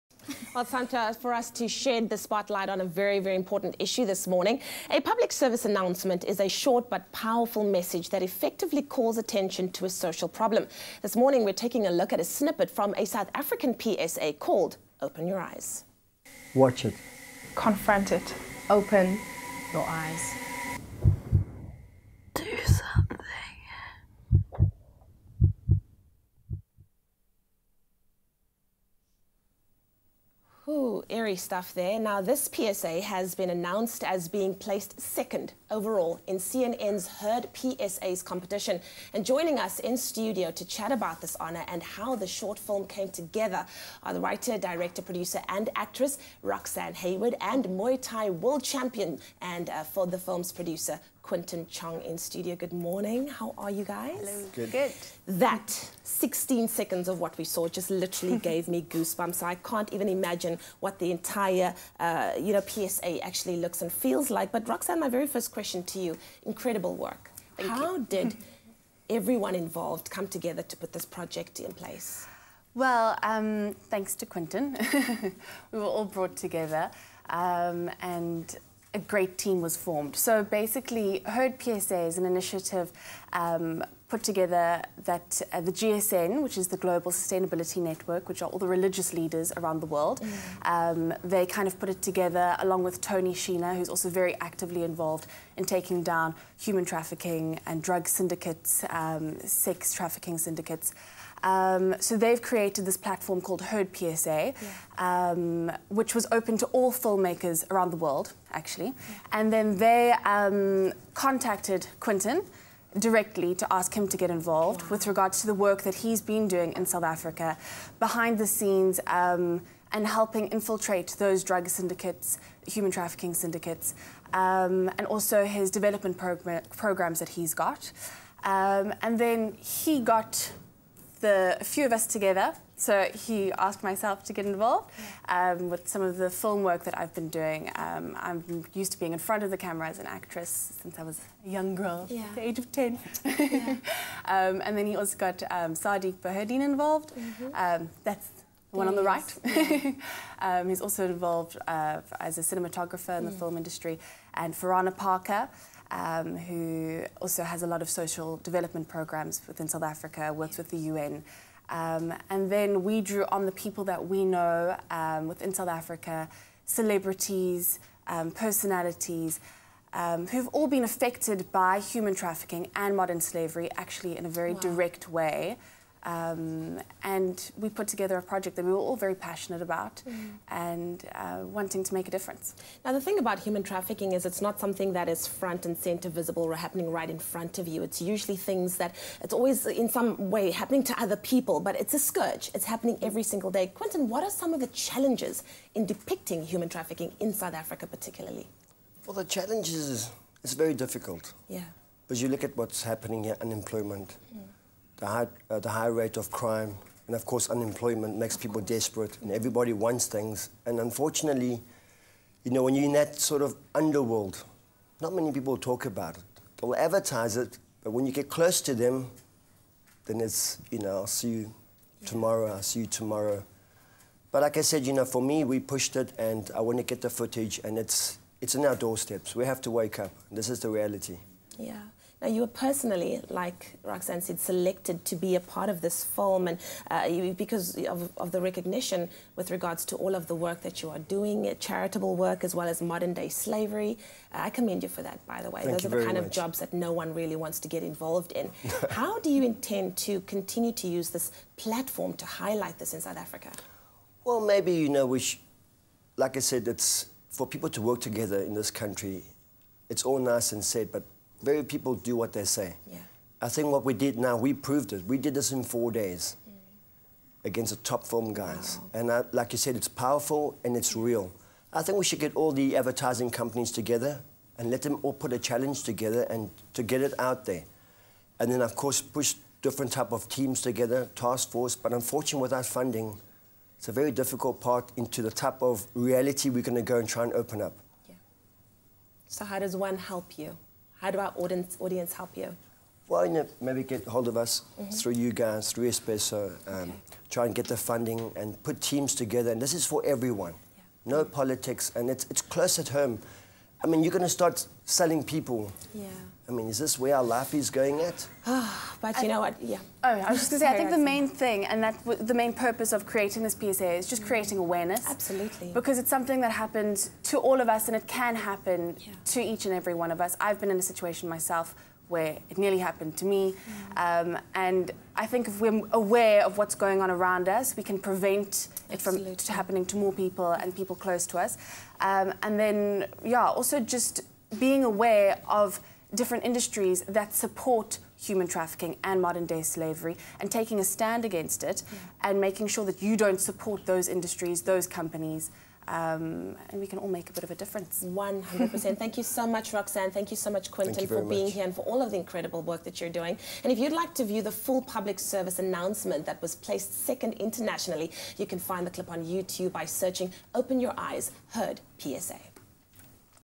'Open Your Eyes' places 2nd in CNN HeardPSA Competition | 4 May 2017
'Open Your Eyes', a South African made PSA that focuses on highlighting issues around human trafficking, has placed 2nd overall in an international competition held by CNN called HeardPSA. The makers of the film, Roxane Hayward and muay Thai world champion Quentin Chong, join Expresso to tell us more about the making of the short film.